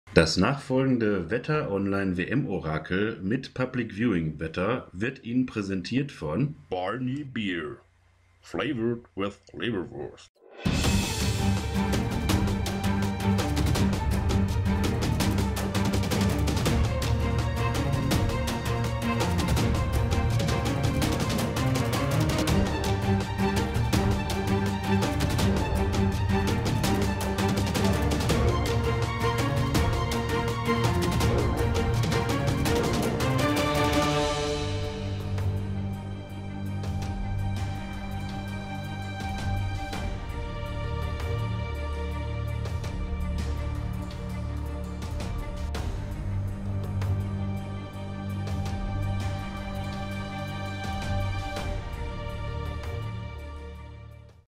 Beagle Barnie WM-Orakel Deutschland vs USA
Wetterhund Barnie orakelt das Spiel - WetterOnline das Public-Viewing-Wetter.